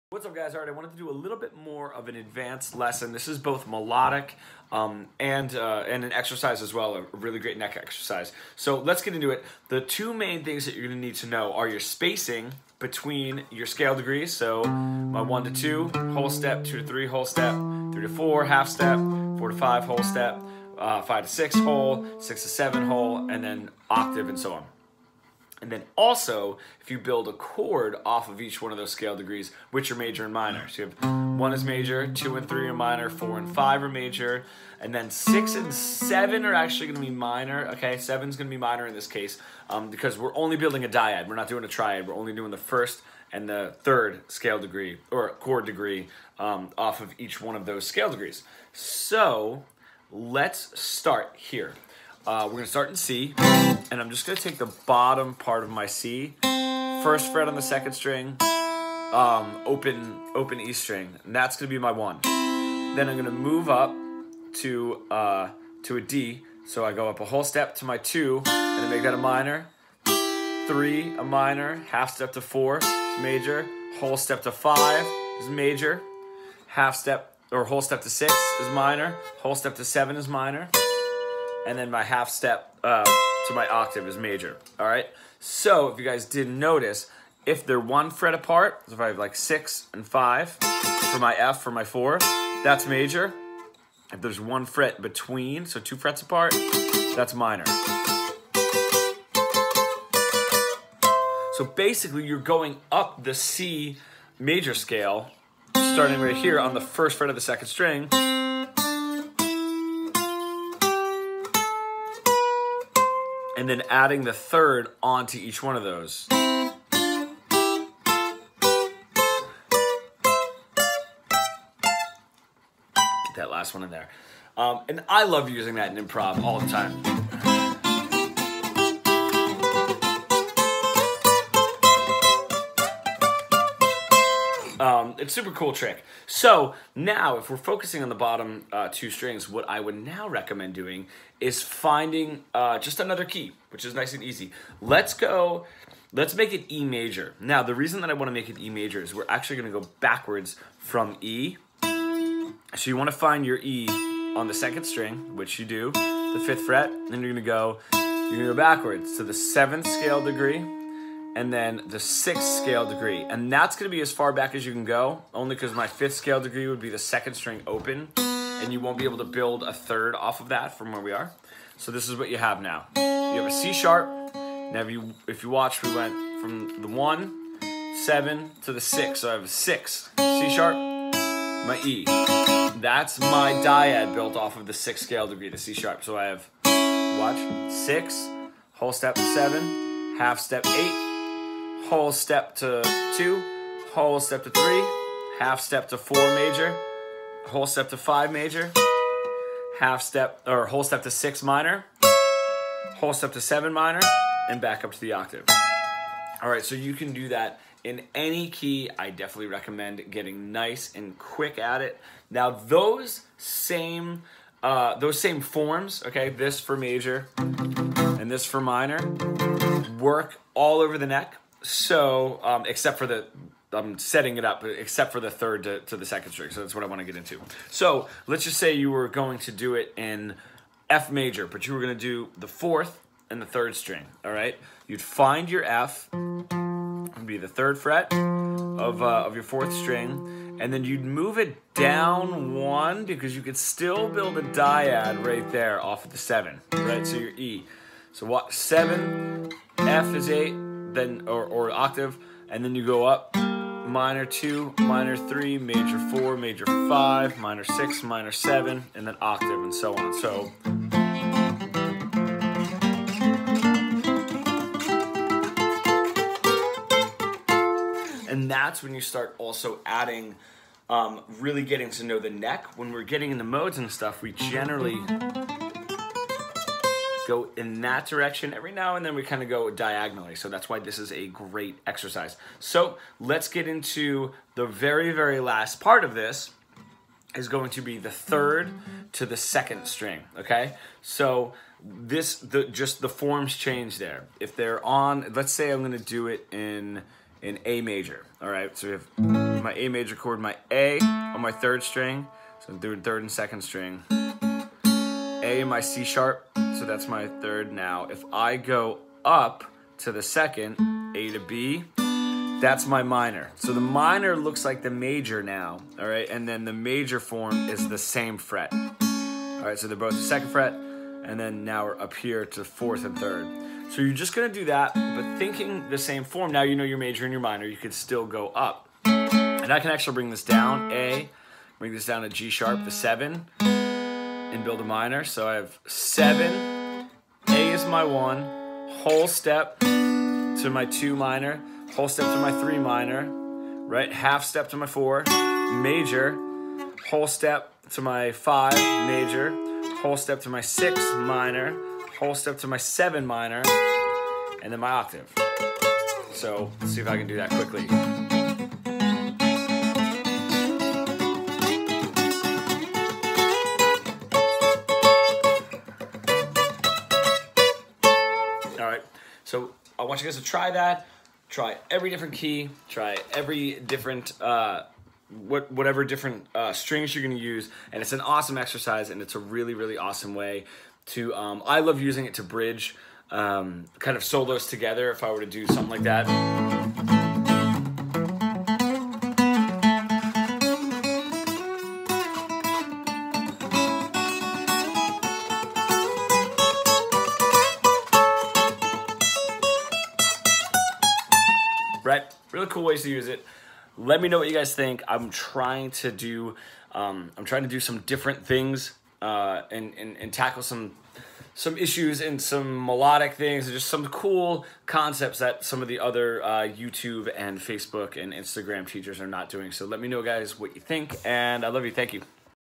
Guitar Exercise - Learning the Neck and Building Melody Together
Here’s a great guitar exercise for bridge learning the neck with melodic building.